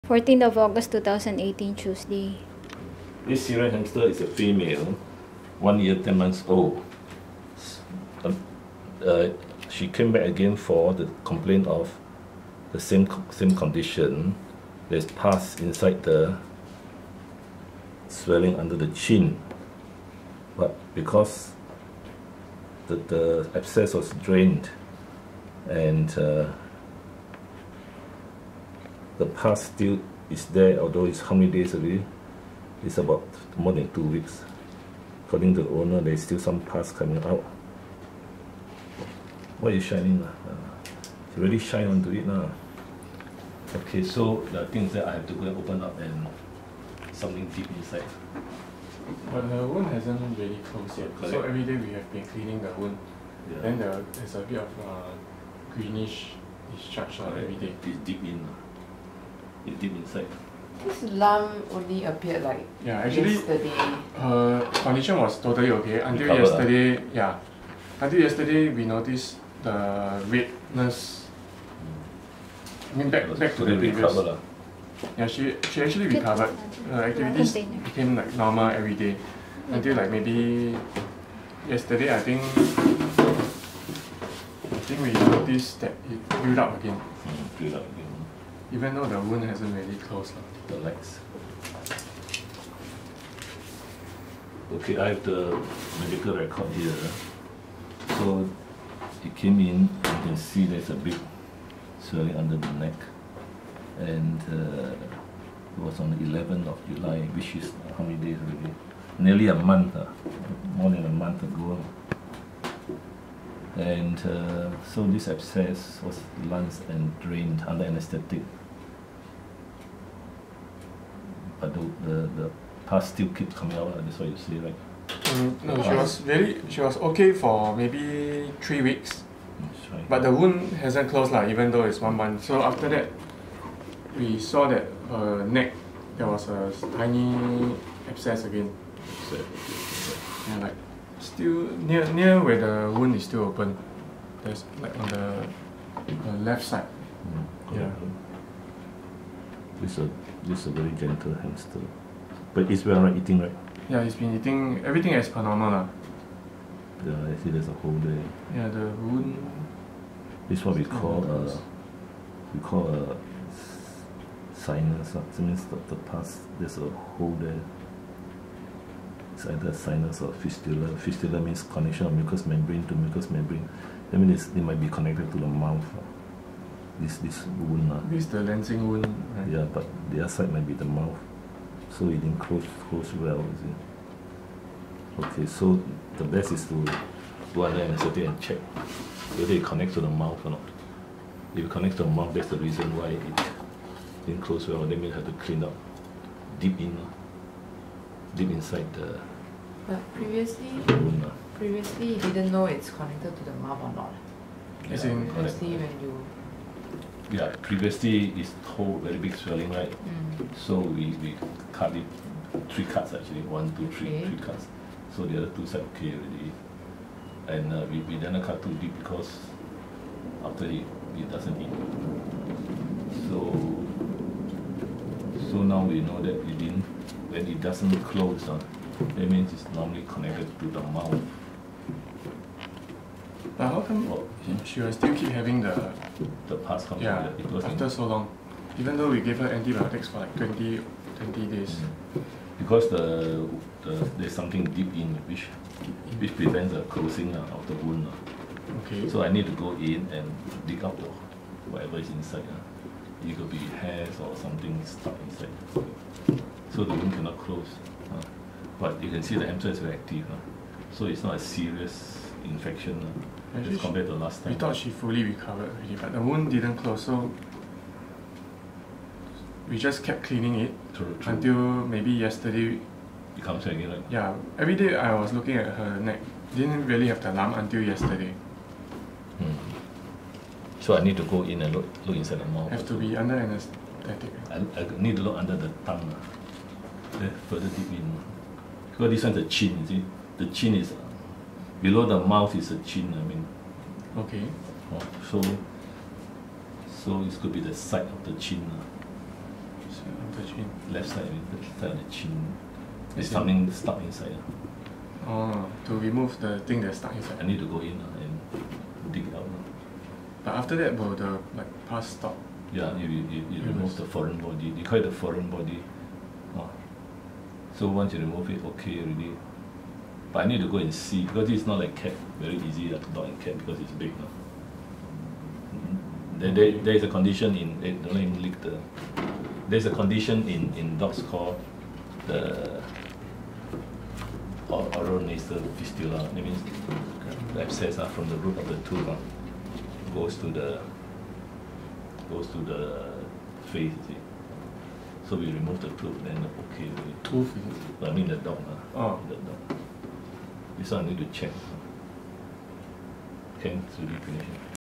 A Syrian Hamster's chin wound will not close for past 4 weeks. Why? Pt 2/4
A non-healing wound for the past 4 weeks. The cause is likely a foreign body stuck inside the wound, preventing closure. Anaesthesia is needed to check on this.

Later videos will show a bone fragment lodged deep inside the wound, preventing non-healing or closure of the wound for the past 4 weeks.

Anaesthesia revealed a bone fragment stuck inside the wound, preventing it from closing. Bone could be from left lower jaw bone breaking up due to front tooth root infections, as in the rabbit's dental problems.  A rare occurrence in hamsters.